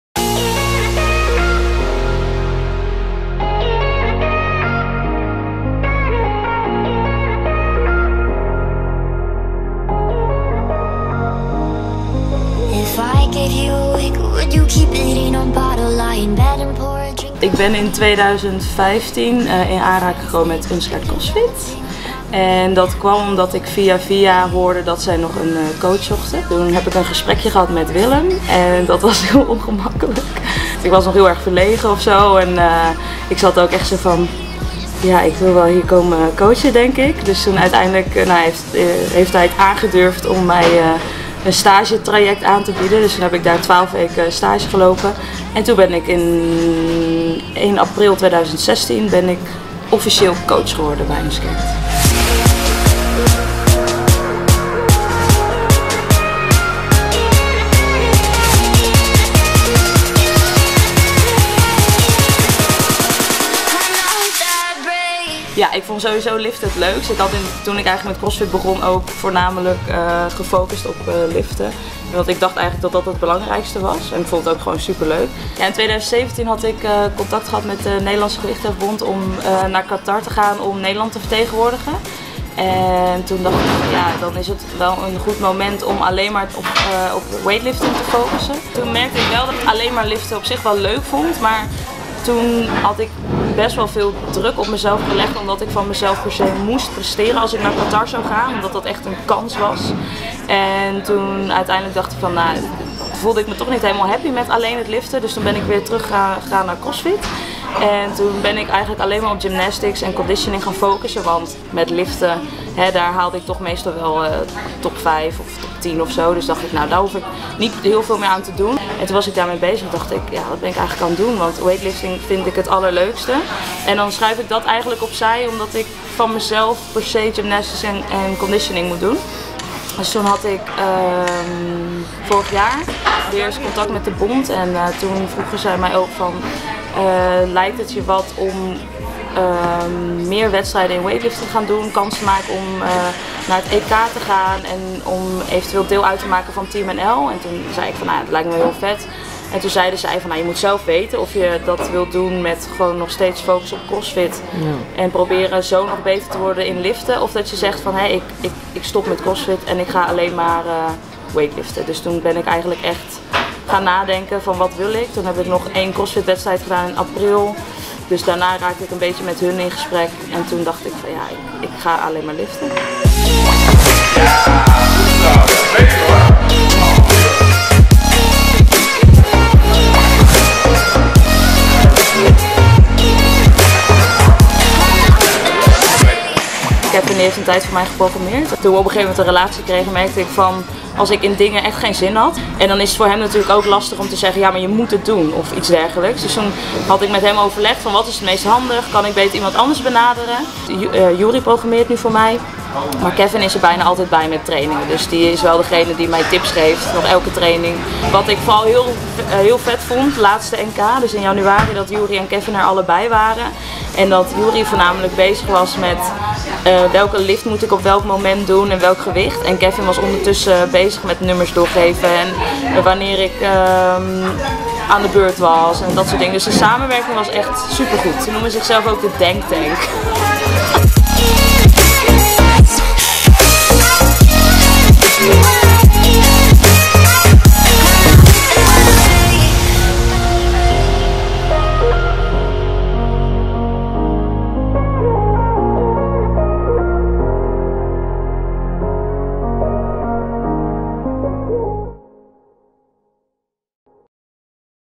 Part 1: Coach Sanne's "Road to Europeans"
Check out the first part of UnScared coach and athlete Sanne Bijleveld's preparation for her first very successful trip to the European Championships in Georgia